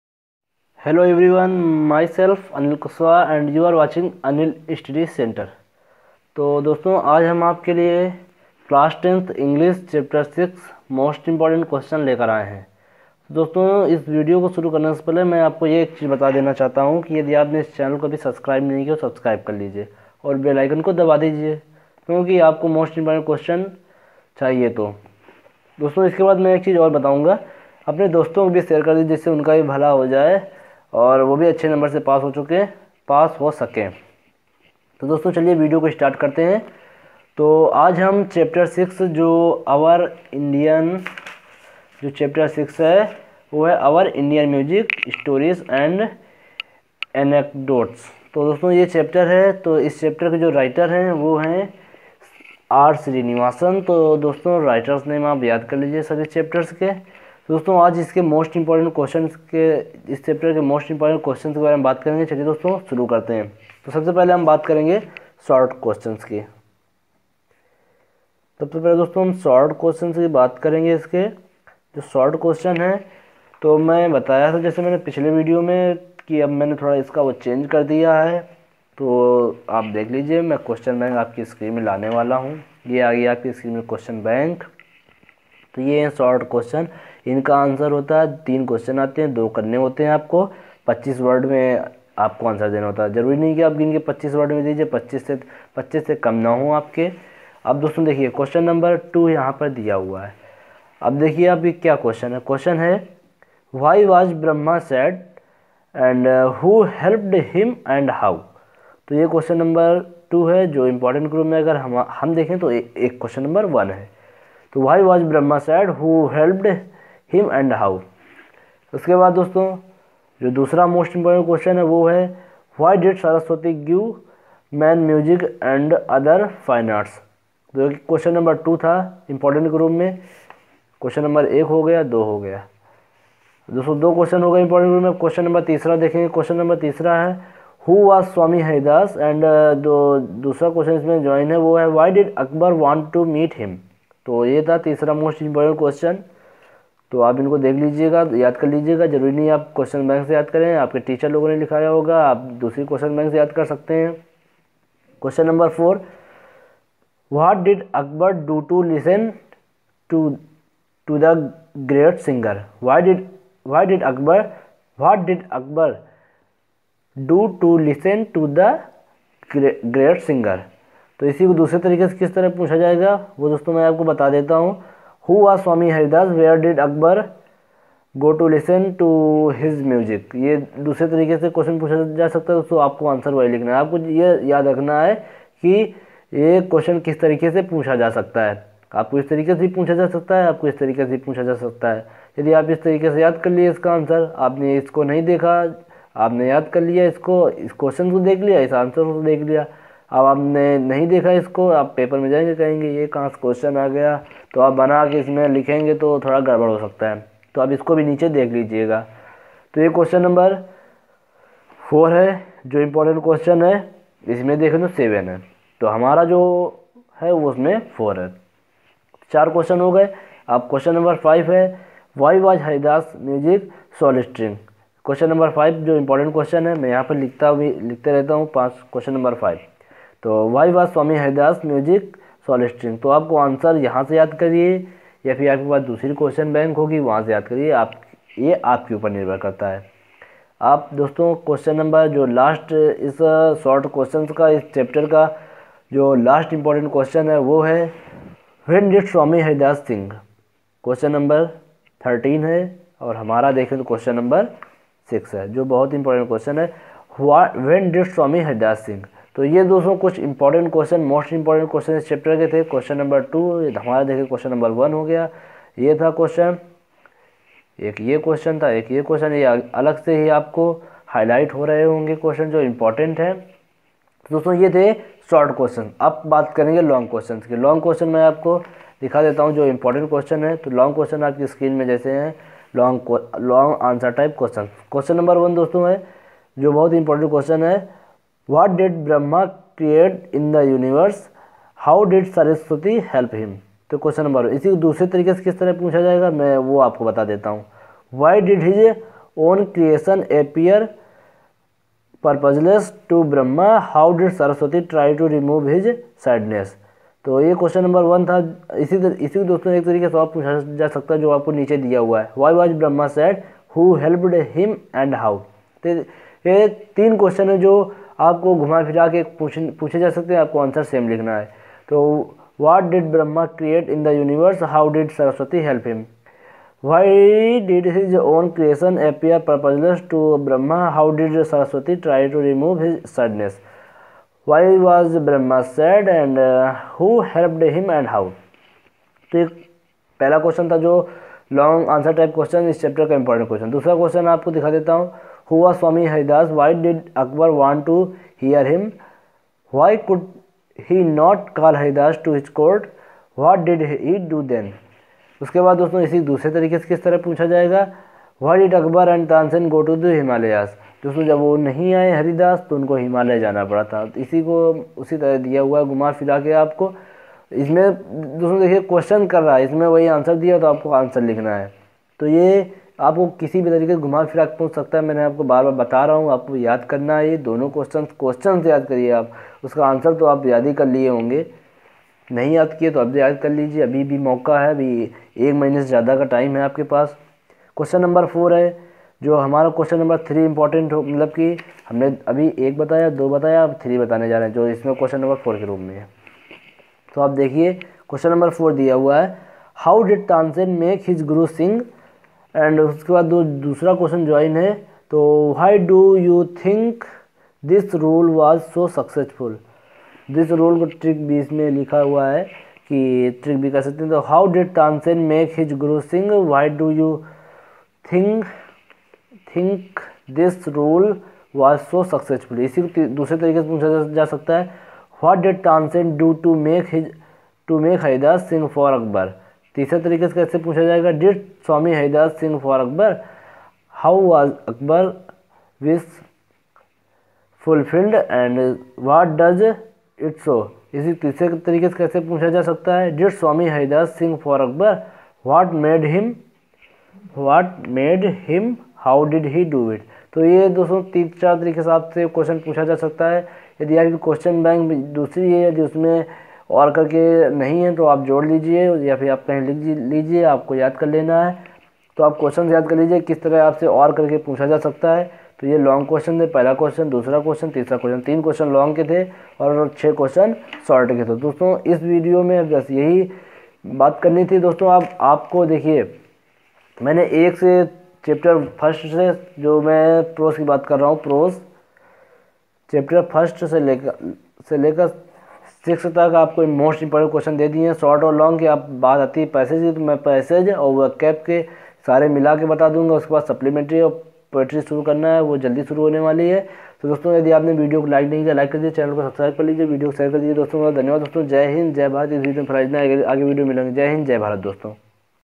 UP board English most important questions chapter-06(Indian music)//यूपी बोर्ड महत्वपूर्ण प्रश्न//
दोस्तों इस वीडियो में मैंने कक्षा 10 अंग्रेजी के महत्वपूर्ण प्रश्न के बारे मे बताया है यदि यह वीडियो आपको पसंद आए तो लाइक कर दीजिएगा और चैनल को सब्सक्राइब कर लीजिएगा